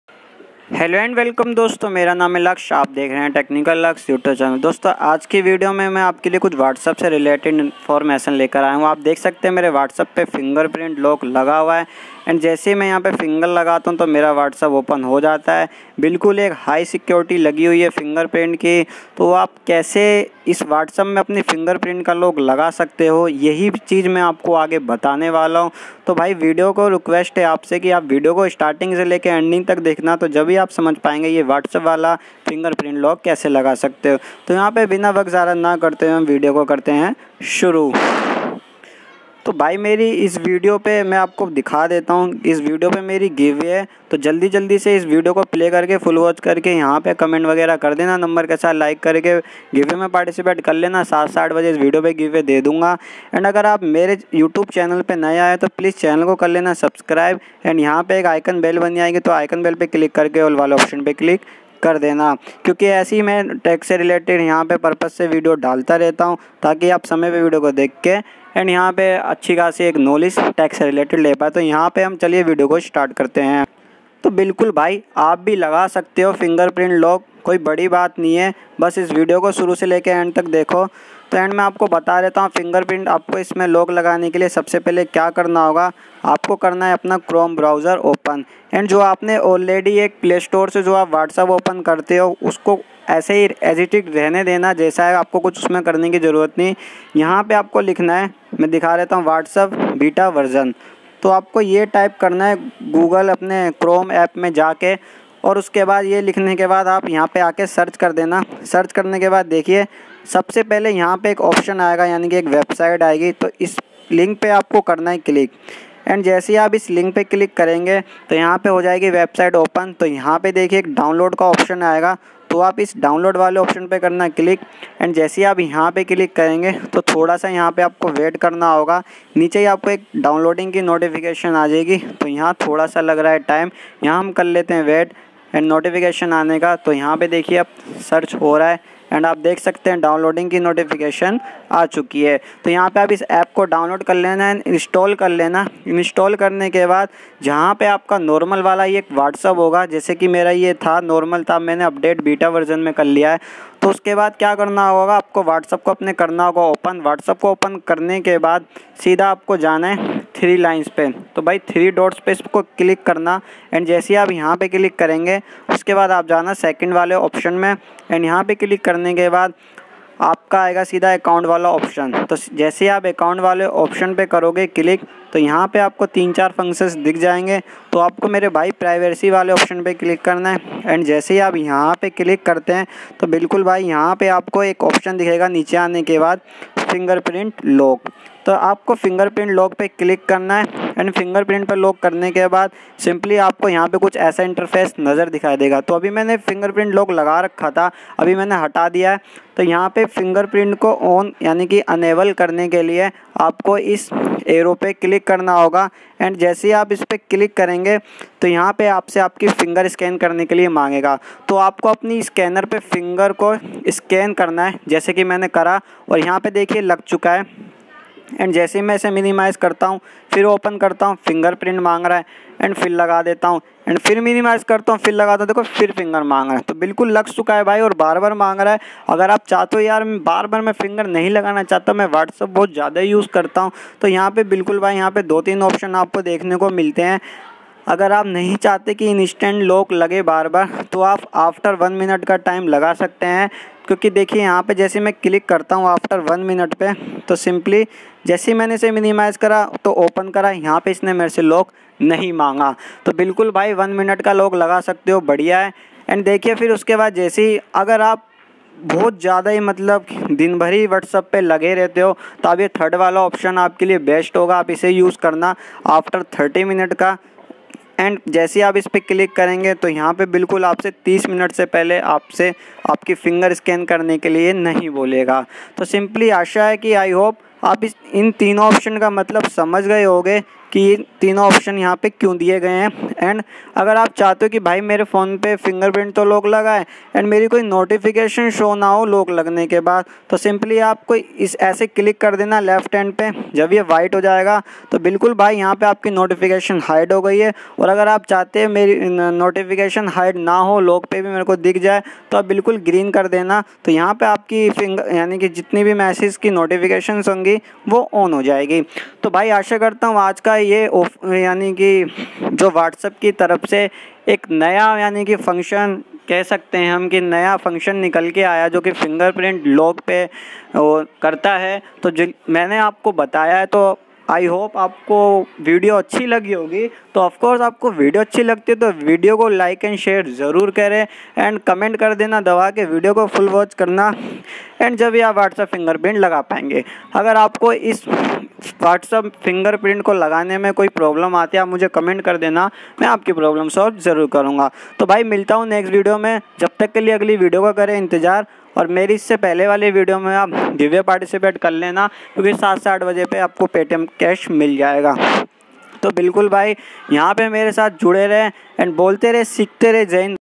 WhatsApp fingerprint lock || WhatsApp fingerprint lock kaise kare
Note : This is an informational

Whatsapp को Unlock कीजिए - Now unlock WhatsApp with your fingerprint
hello friends in this video I am going to tell you about an update which is coming from WhatsApp by which you can unlock your WhatsApp account with your fingerprint.if you want to know about the full details in please watch the full video.I think this video will help you to know more.thanks for watching.